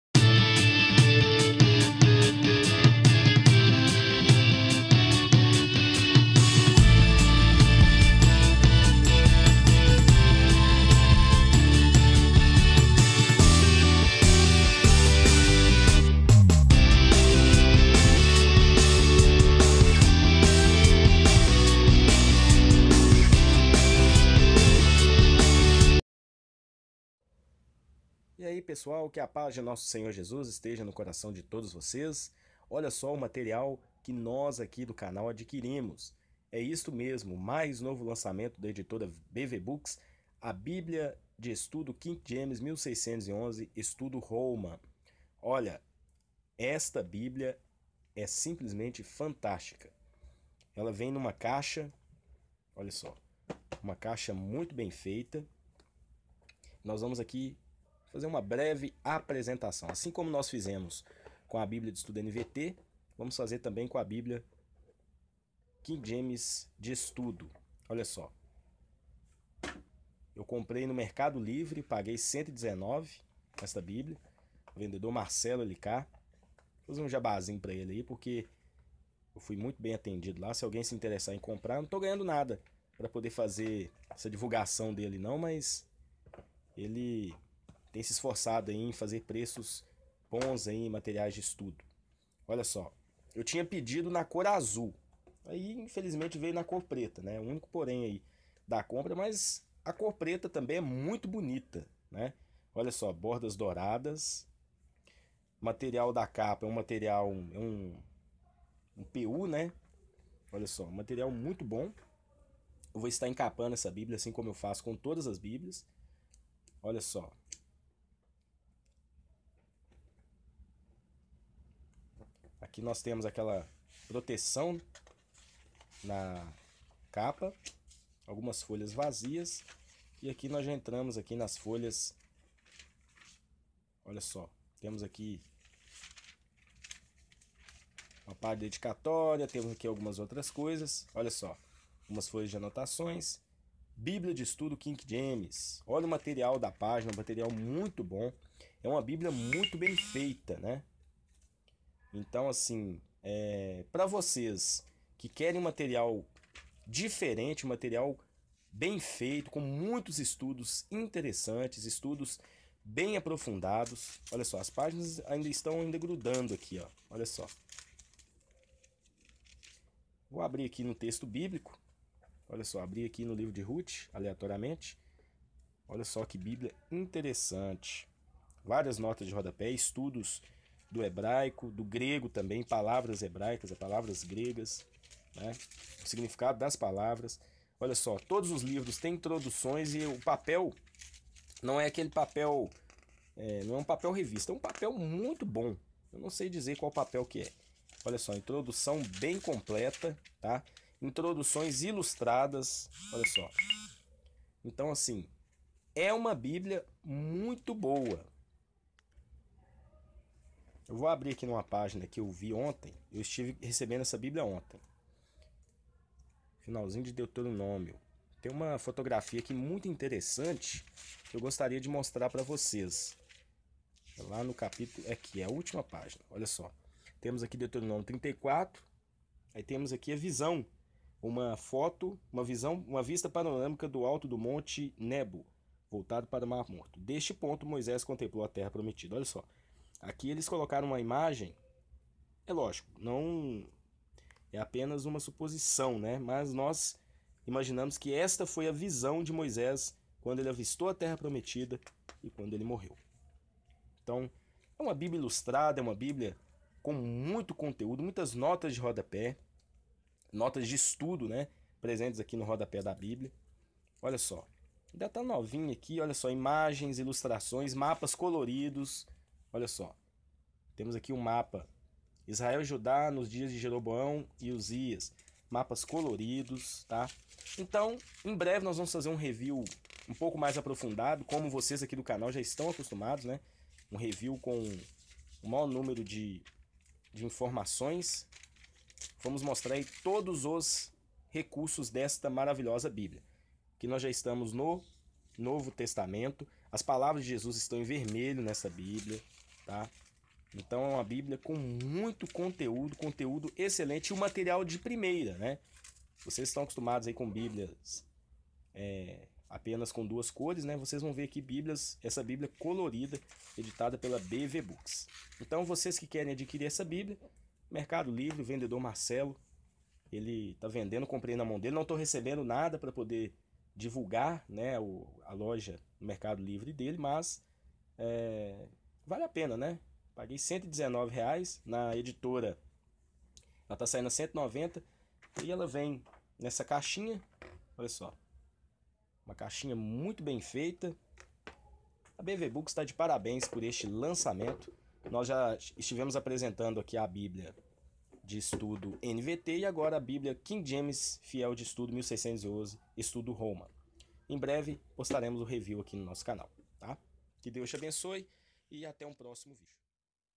Bíblia de Estudo King James Fiel 1611 Estudos Holman
Introdução aos recursos da Bíblia de Estudo King James Fiel 1611 (Estudos Holman). Essa Bíblia, editada pela BV books é simplesmente fantástica! Em breve, o Review completo..